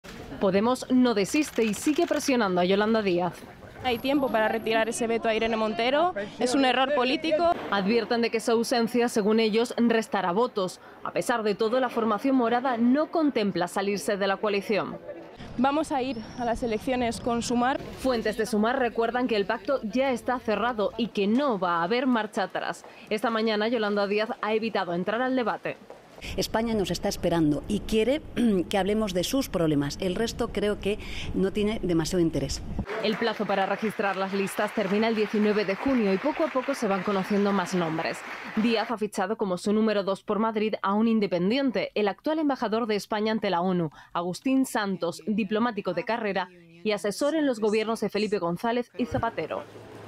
ELECCIONES-SUMAR: YOLANDA DÍAZ sobre el "VETO" a IRENE MONTERO: "No tiene interés" | RTVE
Con las coaliciones ya registradas para las elecciones generales del 23J, toda la atención está puesta ahora en las listas. Quedan siete días para que los partidos decidan qué nombres llevarán en sus papeletas.

Podemos insiste en que se incluya a Irene Montero pero Yolanda Díaz, que ya tiene a su número 2, da por zanjada la cuestión. Será el actual embajador de España ante Naciones Unidas, Agustín Santos Maraver, diplomático de carrera desde 1982.